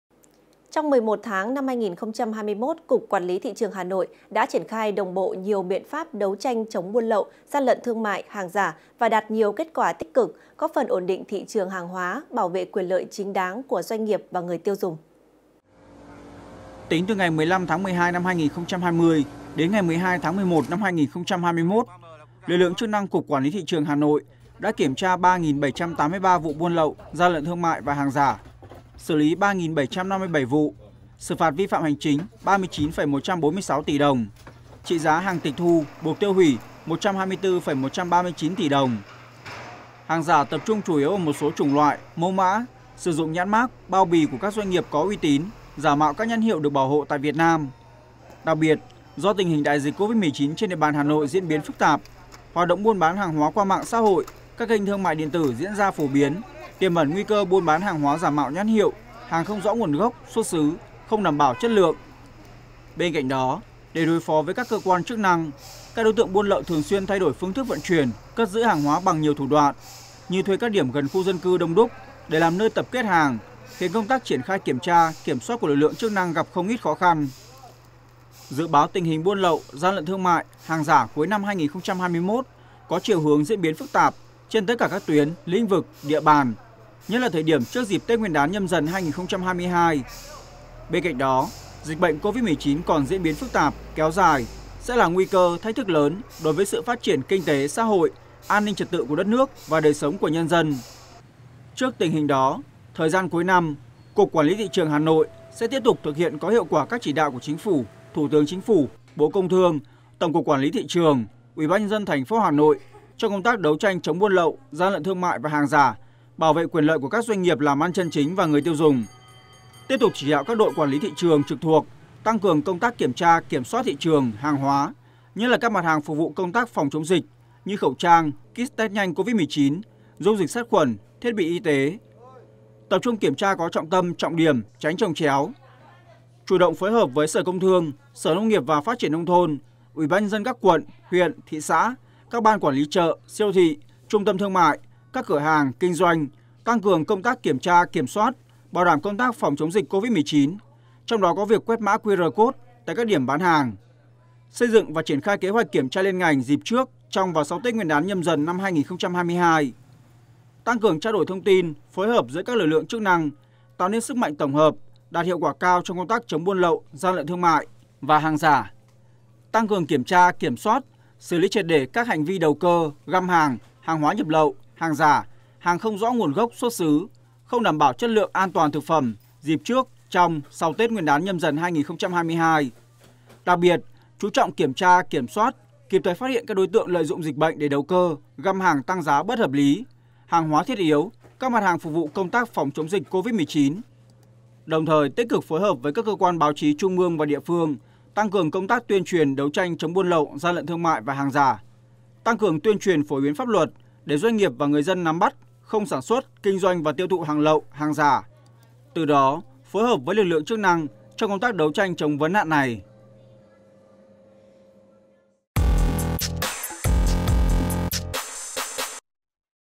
Tiếp tục tăng cường kiểm soát thị trường dịp cuối năm
Cục Quản lý thị trường HN đã triển khai đồng bộ nhiều biện pháp đấu tranh chống buôn lậu, gian lận thương mại, hàng giả và đạt nhiều kết quả tích cực, góp phần ổn định thị trường hàng hóa, bảo vệ quyền lợi chính đáng của doanh nghiệp, người tiêu dùng.